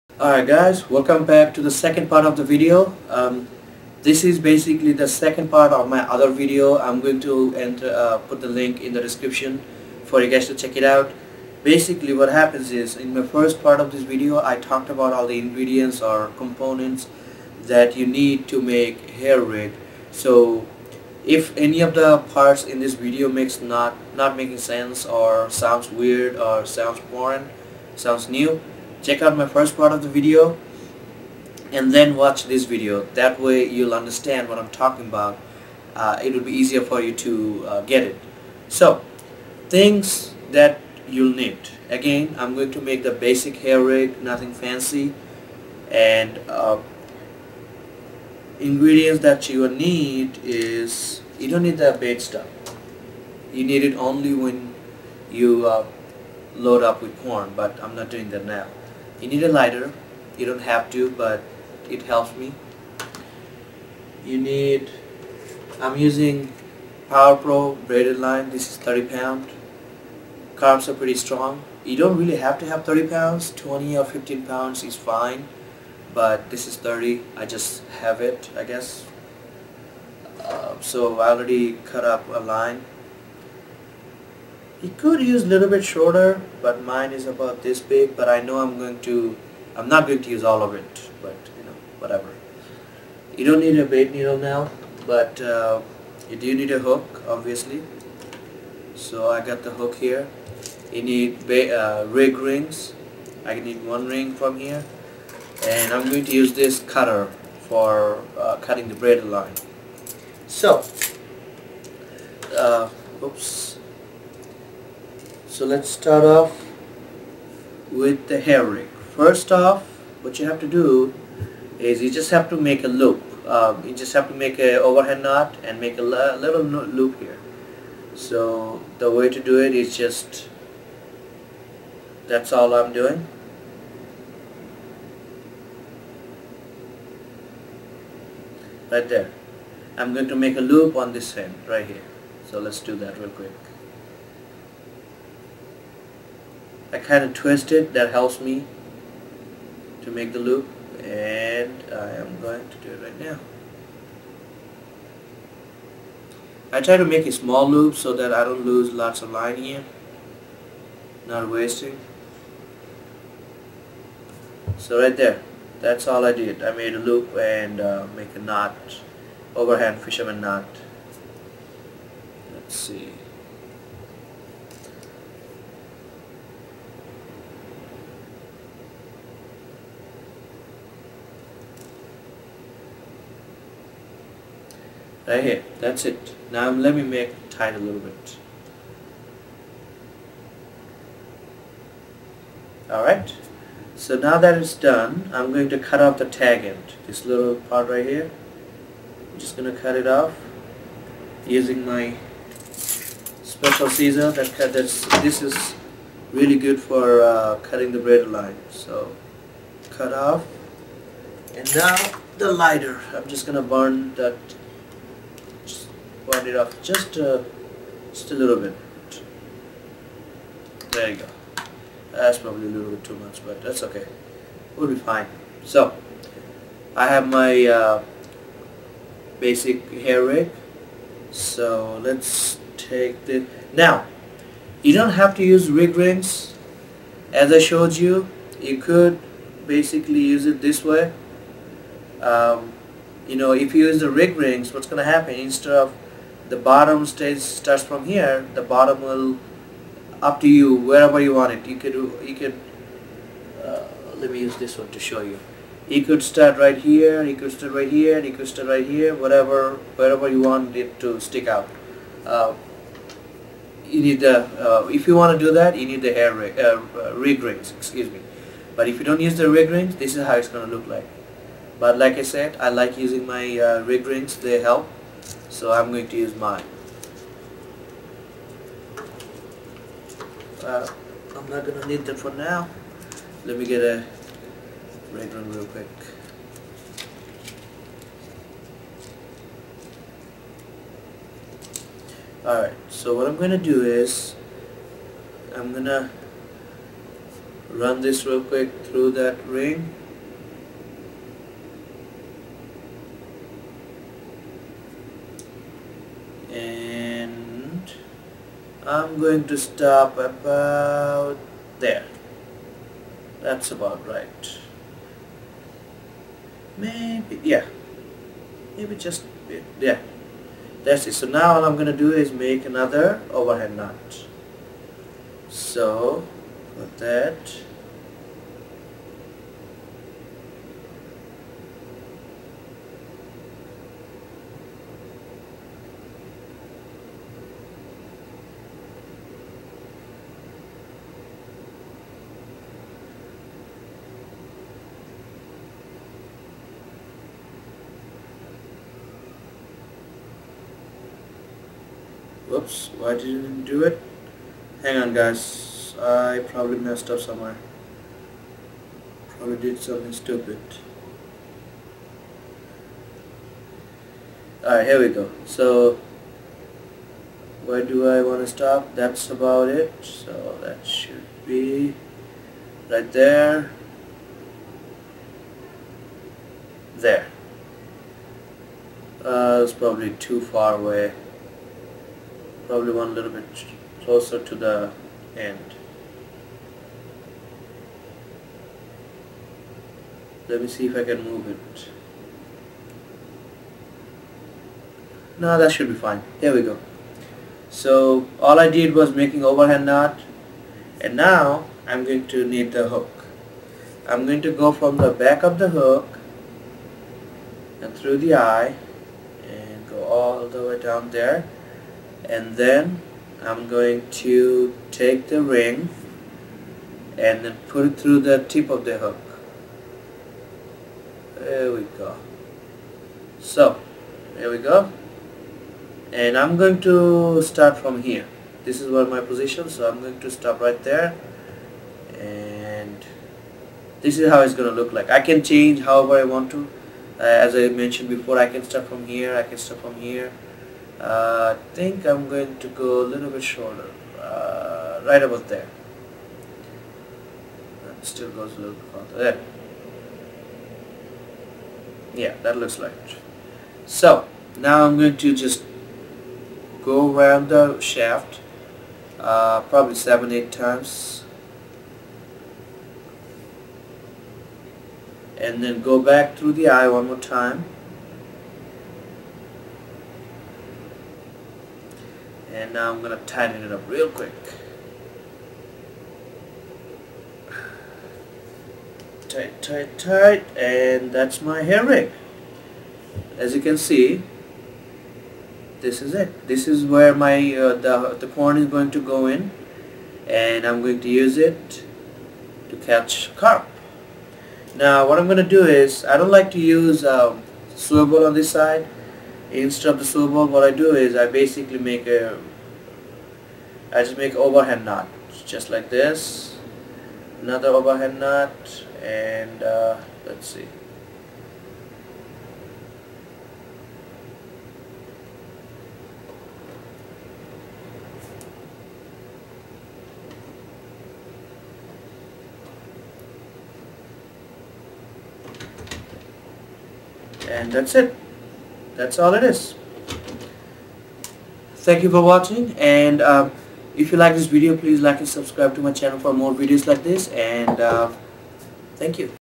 What is Hair Rig and how to make it- Part 2
So I am trying to make a video about Hair Rig and all the details about it. I tried to make it as easy as I could. But if still things dont make sense, lemme know. I will try to work on it.
Link for the first part of this video - goo.gl/QqmcTz